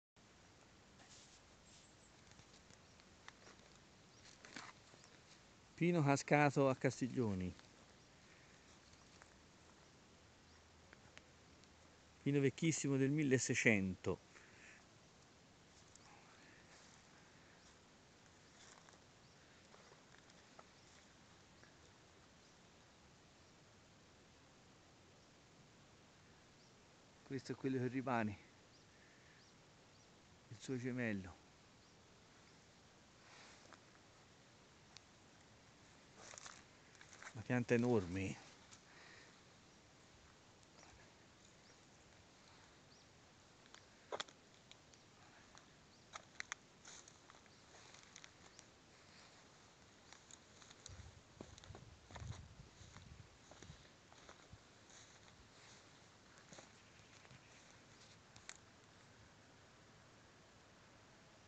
Castiglione della pescaia cade pino secolare.
Pino secolare del 1660 caduto
Diametro 5,35 metri.